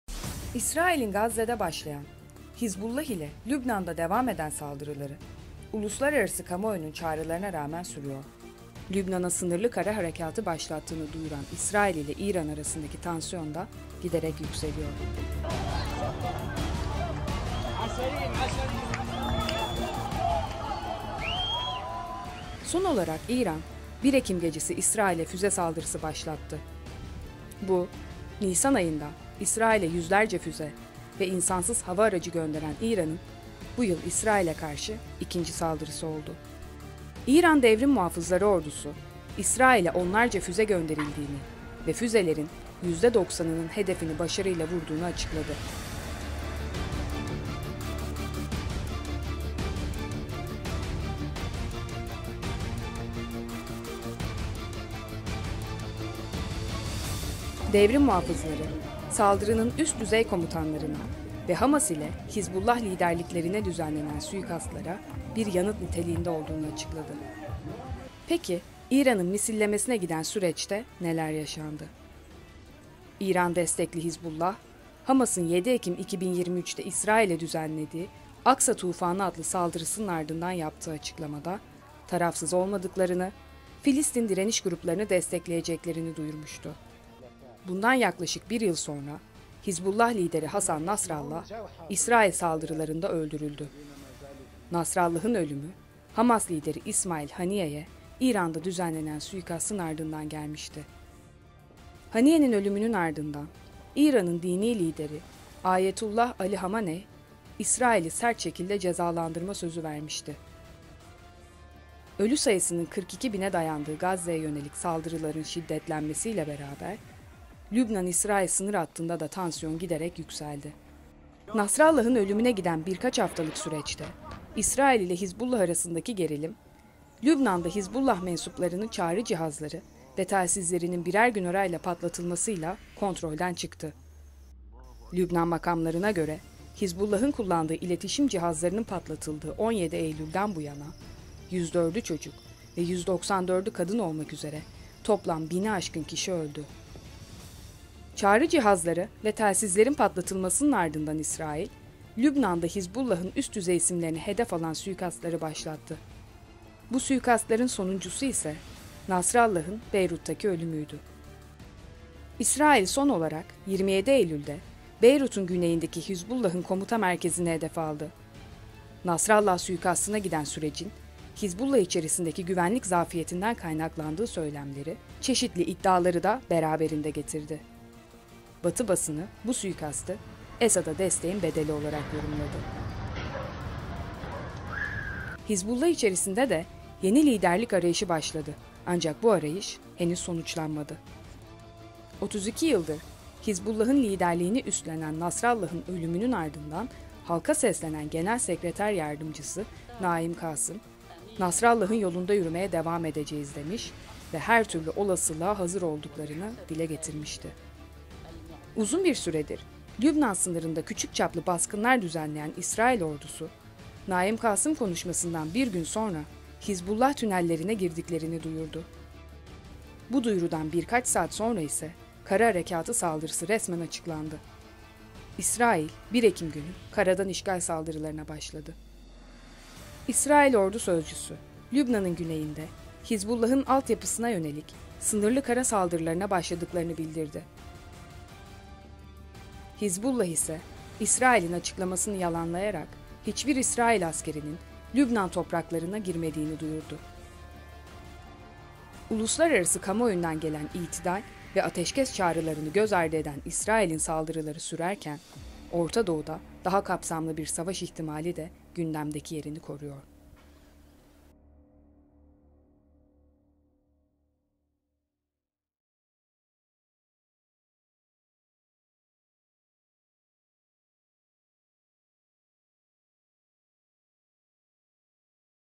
Suikast zincirinin son halkası: Nasrallah'ı ölüme götüren süreç nasıl gelişti?
32 yıldır Hizbullah’ın liderliğini üstlenen Nasrallah'ın suikastına giden sürecin, Hizbullah içerisindeki ‘güvenlik zaafiyetinden’ kaynakladığı söylemleri, çeşitli iddiaları da beraberinde getirdi.

İsrail’in Gazze’de başlayan, Hizbullah ile Lübnan’da devam eden saldırıları, uluslararası kamuoyunun çağrılarına rağmen sürüyor. Lübnan’a ‘sınırlı’ kara harekatı başlattığını duyuran İsrail ile İran arasındaki tansiyon da giderek yükseliyor. Son olarak İran, 1 Ekim gecesi İsrail’e füze saldırısı başlattı. Bu, nisan ayında İsrail’e yüzlerce füze ve insansız hava aracı gönderen İran’ın bu yıl İsrail’e karşı ikinci saldırısı oldu.